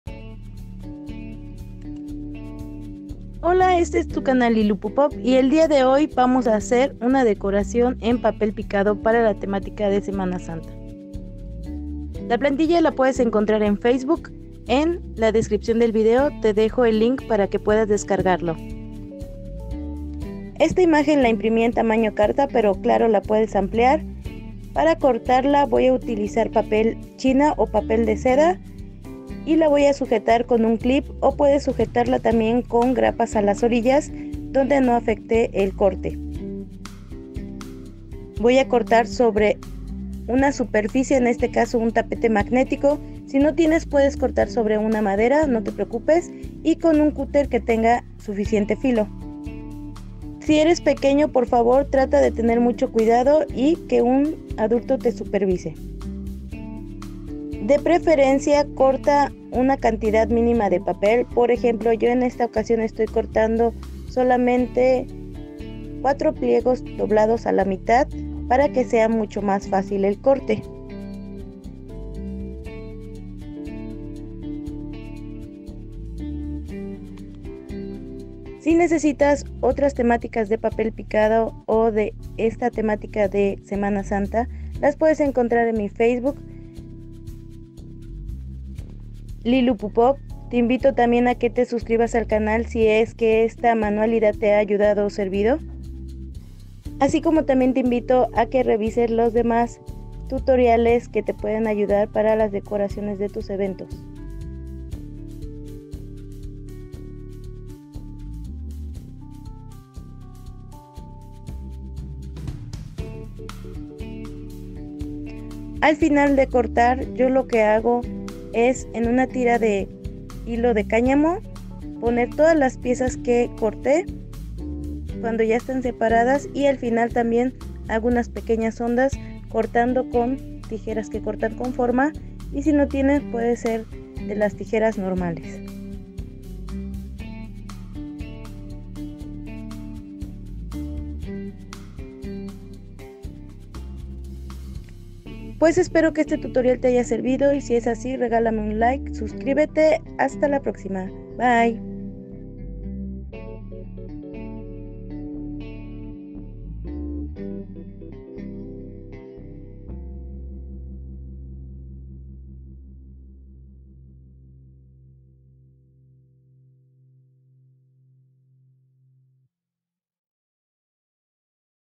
decoracion semana santa 2022 / como hacer papel picado
Aprende a picar papel para esta fecha especial de semana santa con esta plantilla,  usa las medidas pertinentes al cortar.
Estos son materiales:
Base para cortar
La plantilla la tendras que imprimir en unas hoja tamaño carta  y la puedes encontrar en mi facebook

papel china 4 pliegos color que gustes
Cutter con bastante filo si es nuevo mejor
engrapadora y grapas
hilo para hacer la guirnalda
ahi podras encontrar las plantillas de todos mis tutoriales, TODAS LAS PLANTILLAS SON GRATIS, los encontraras en los albunes de fotos